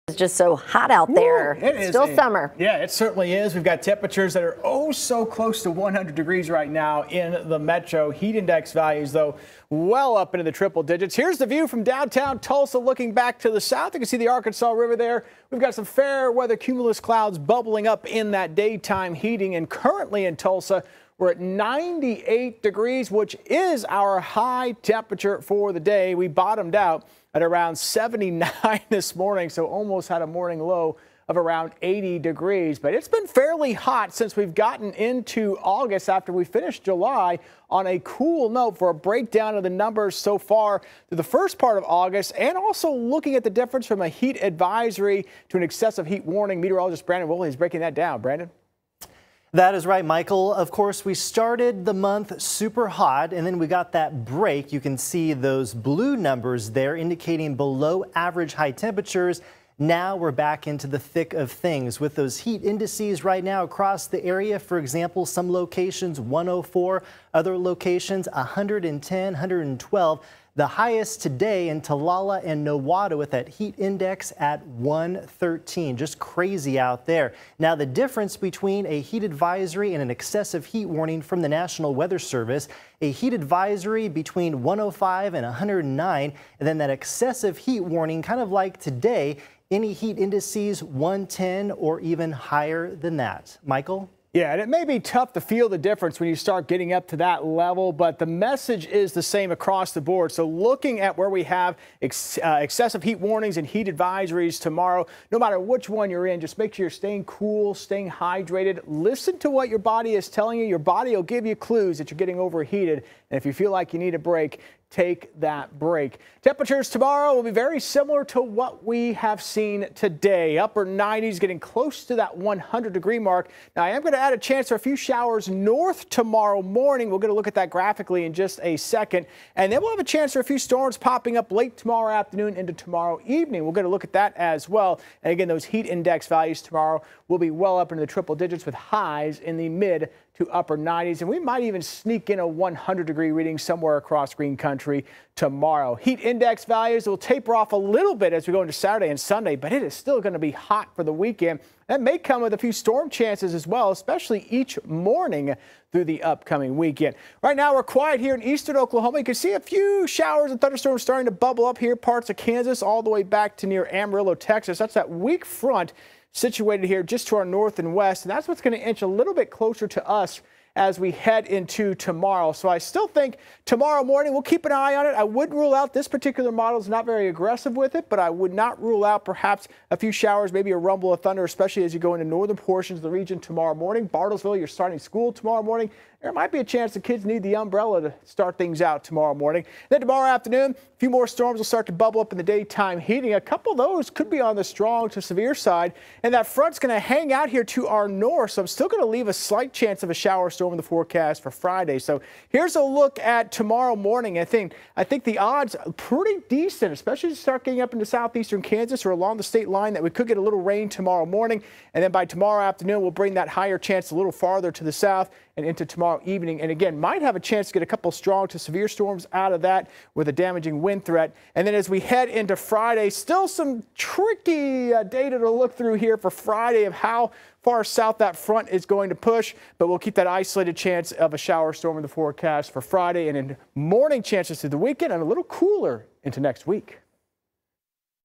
Dangerous heat and a few storms Thursday
Hot and humid conditions persist Thursday with excessive heat warnings and heat advisories. After a morning rain chance north, a few strong to severe storms are possible in the afternoon/evening.